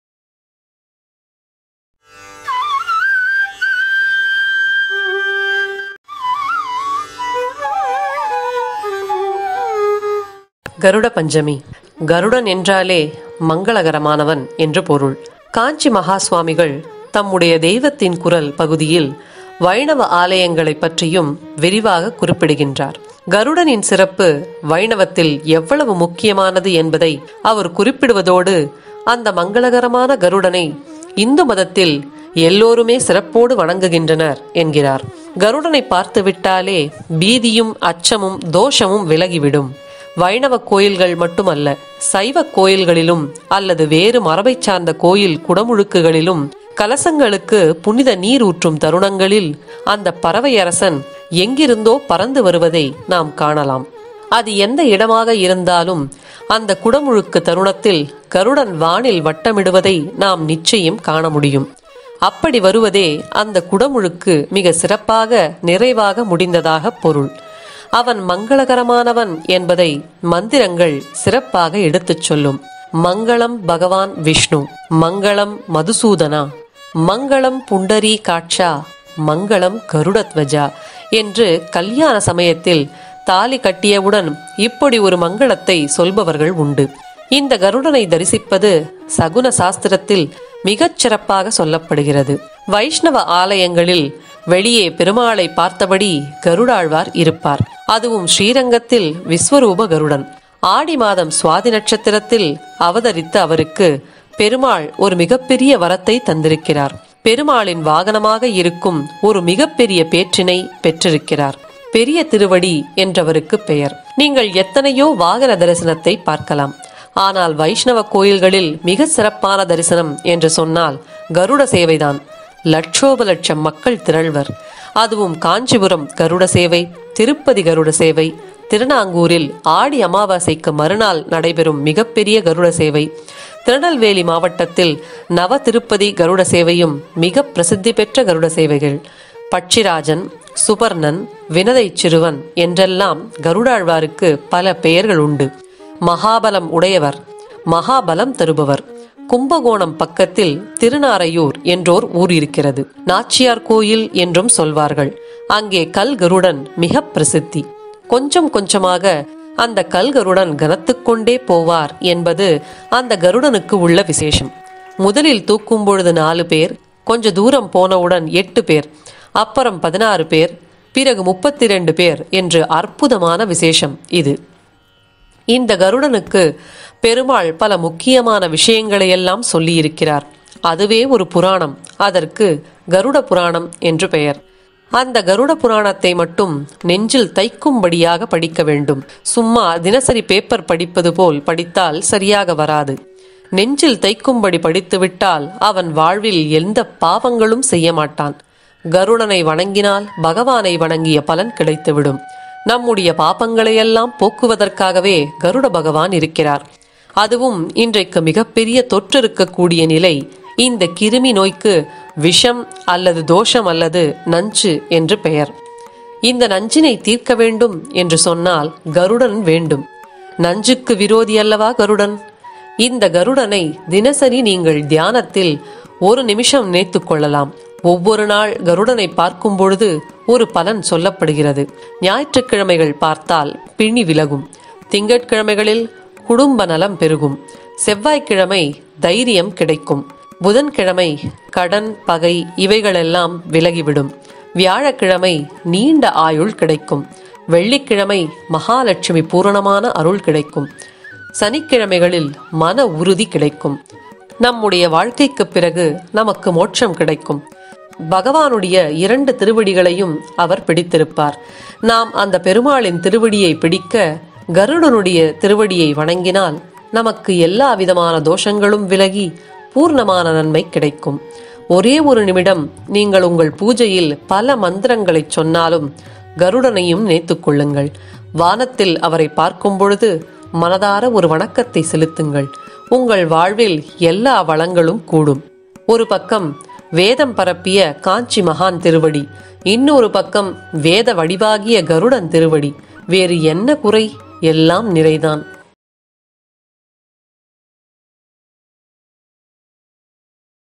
கிருமி நோய் பரவிய இந்த வேளையில் தினசரி பூஜையில் ஒரு நிமிஷம் இவரை நினைக்கவும்- Mahaperiyava - Garudan
மஹா பெரியவா அருளால் வாழ்க்கை எப்படி மாறியது?